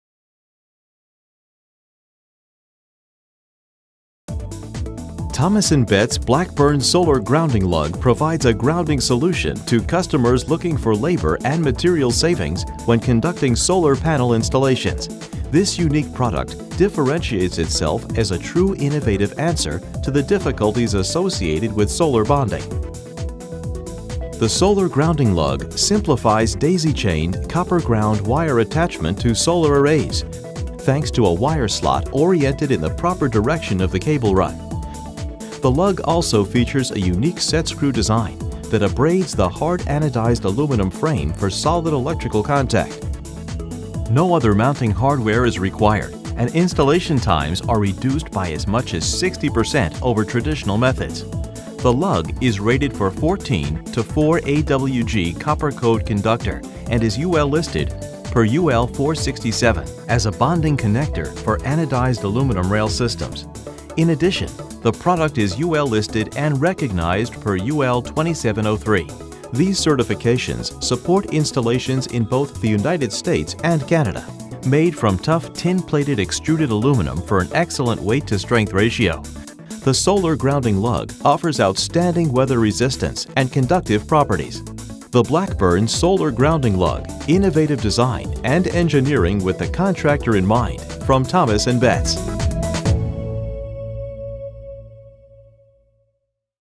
Blackburn® Solar Grounding Lug
The Blackburn® Solar Grounding Lug provides an excellent grounding solution for customers desiring labor and material savings during solar panel installation.